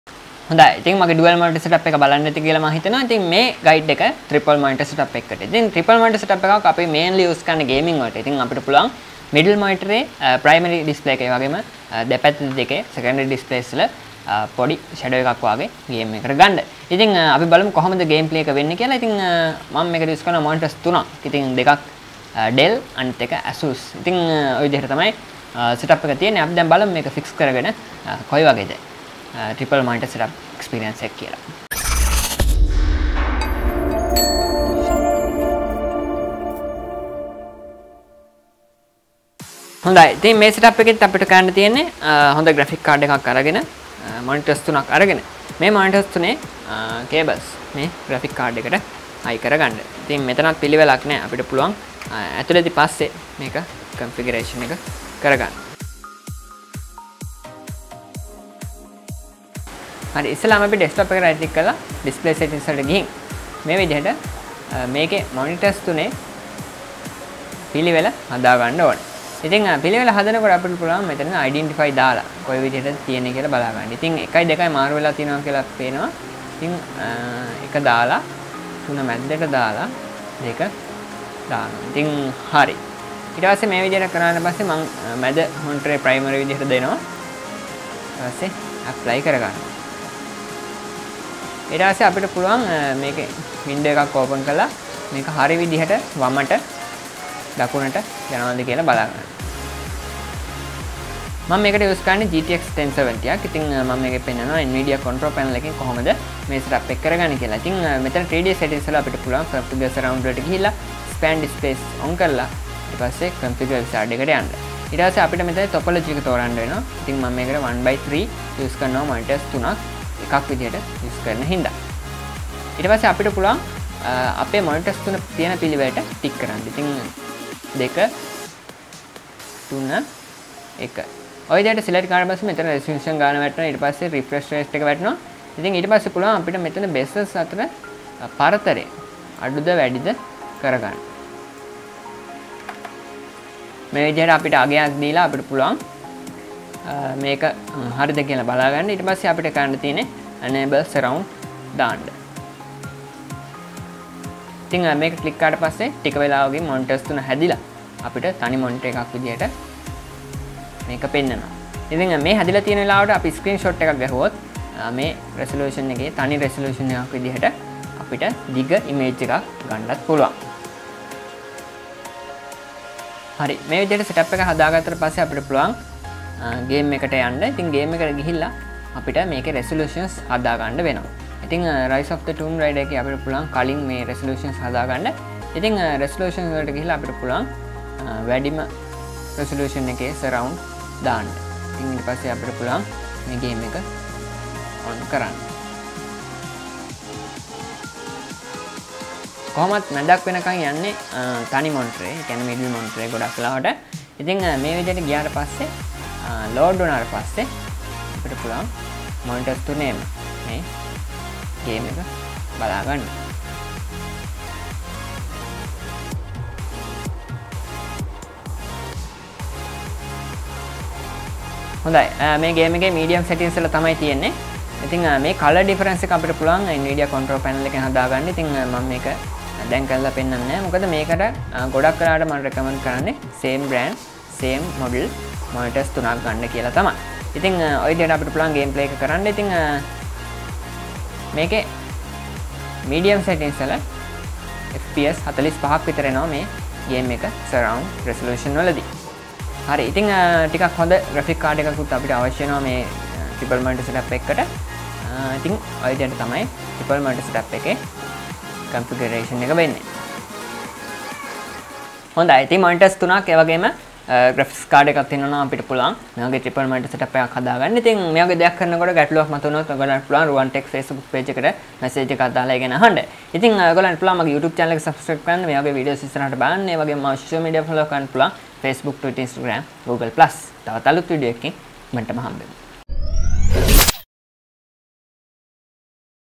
Triple Monitor Setup Guide in Sinhala Sri lanka
සිංහල Tech Show Sri Lanka by Ruwan Prasanna

Asus 24" IPS 23,000/=
Dell 23" TN 18,000/=

Sounds not well because raining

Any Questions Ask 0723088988
Viber / WhatsApp / SMS